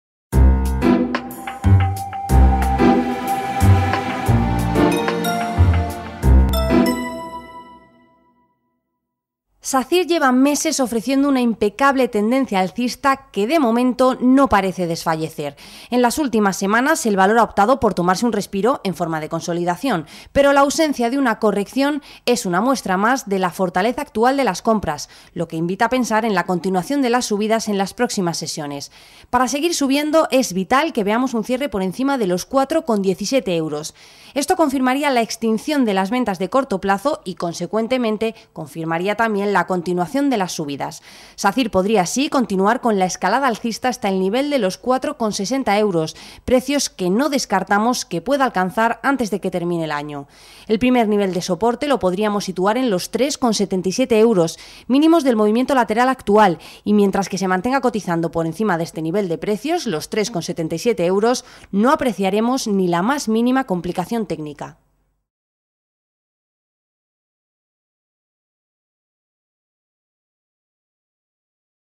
Análisis técnico de Sacyr
Es vital que veamos un cierre por encima de los 4,17 euros lo que confirmaría la extinción de las ventas del corto plazo y consecuentemente la continuación de las subidas.